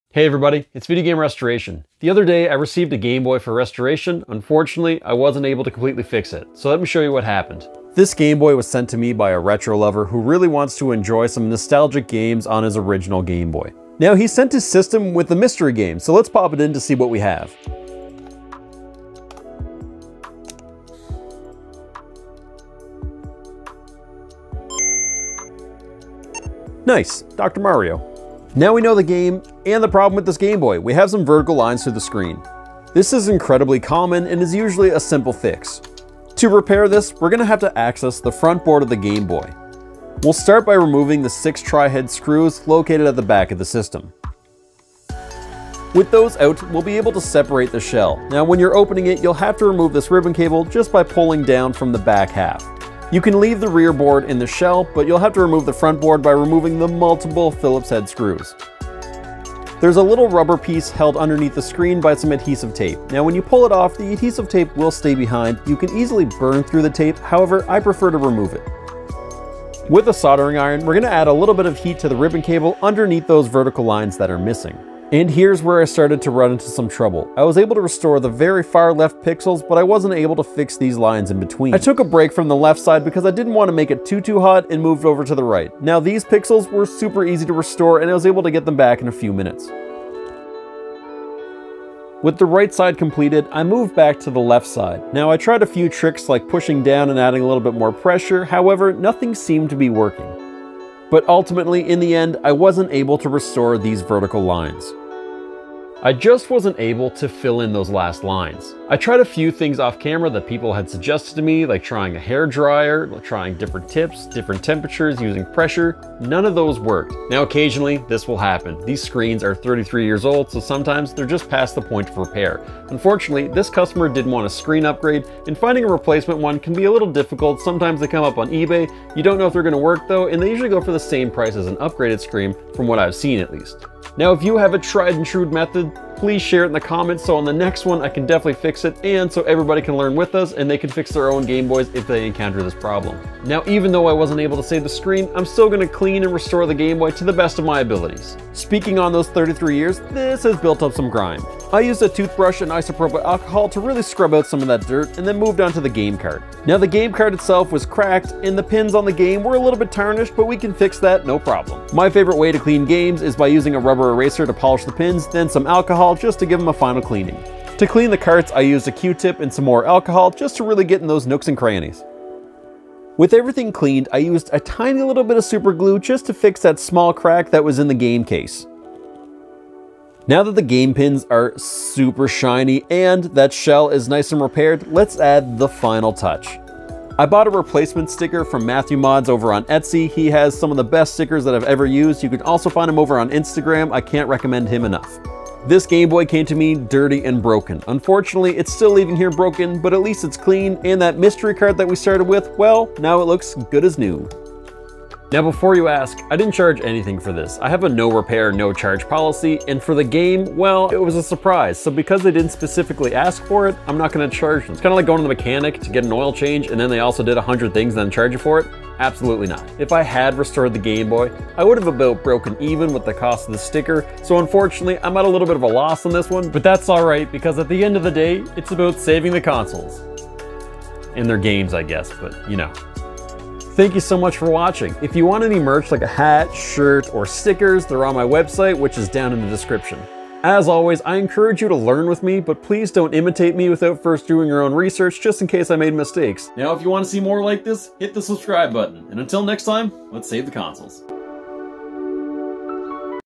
I Couldn't Save This Gameboy
Sometimes things are just past the point of repair 😔 At least something was saved!

See anything you would have done differently? let me know! I'm self taught and learning more each day I'll take any lessons I can get!